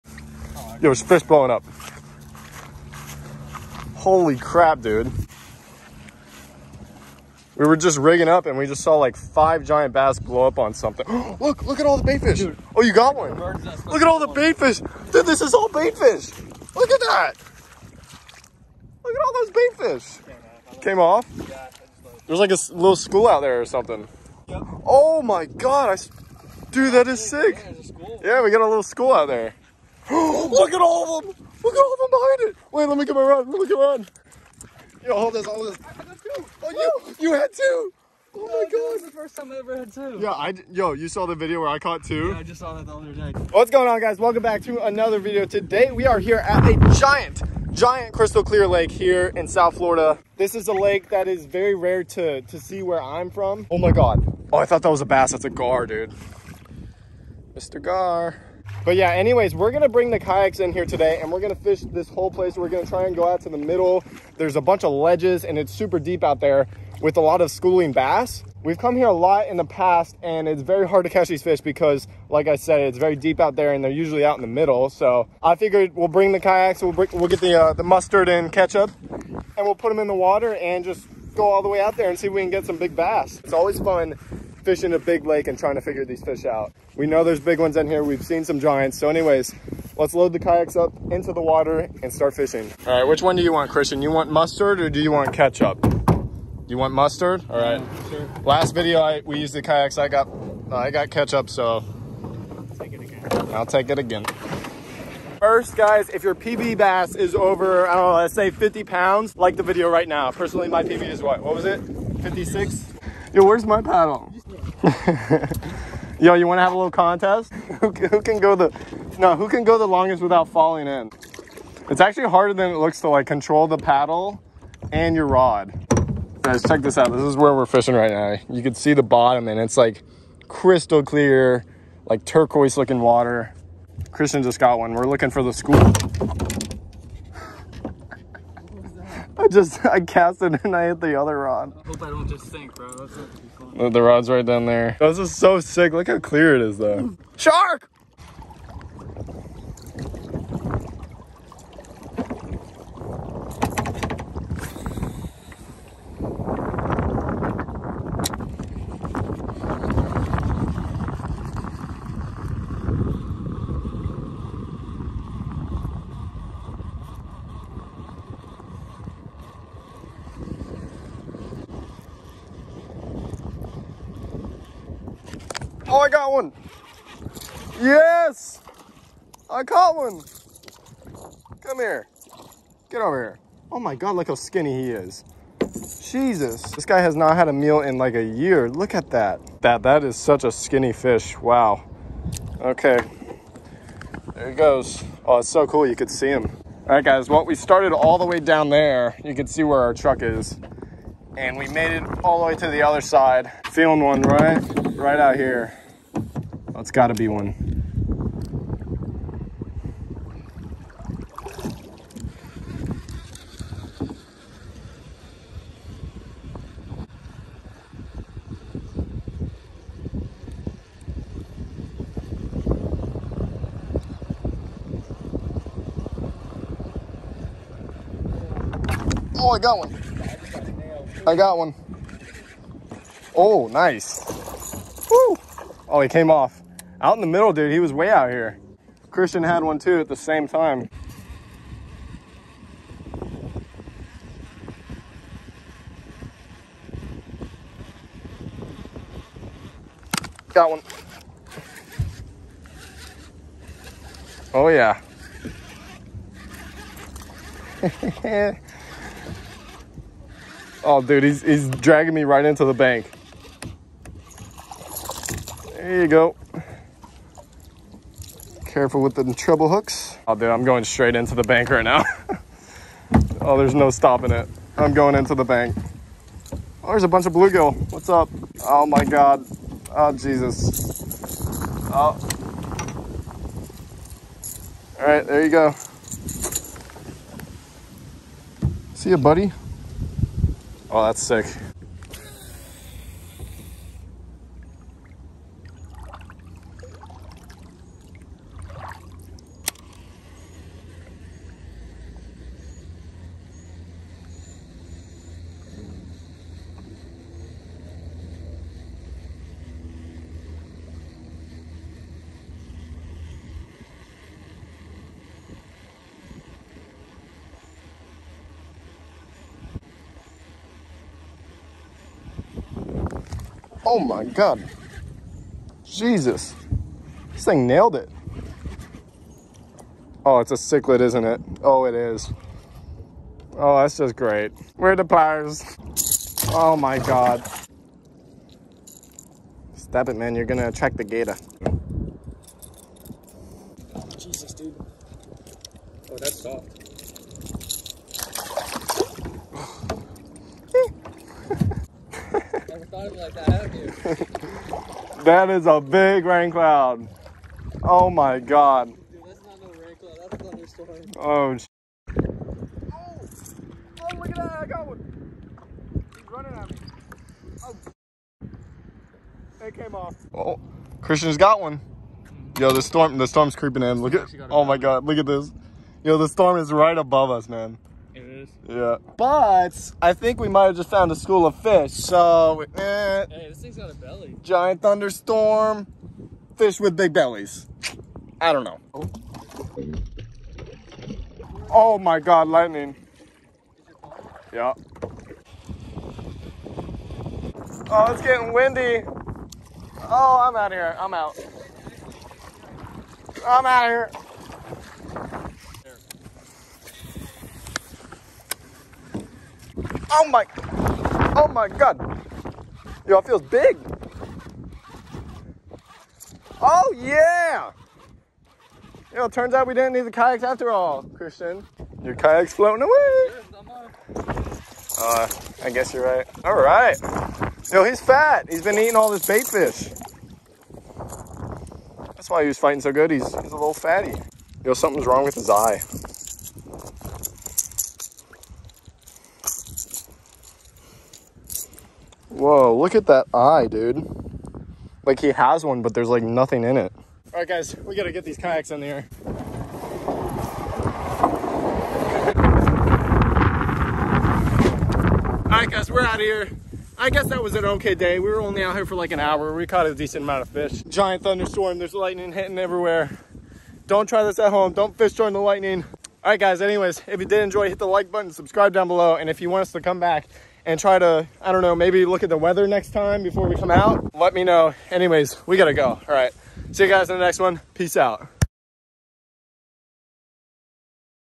Fishing CRYSTAL CLEAR Lake with Kayaks!!!
In today’s video, we go kayak fishing in a crystal clear lake in south Florida, our target is to find a school of largemouth bass. Things go south when a giant thunderstorm rolls in!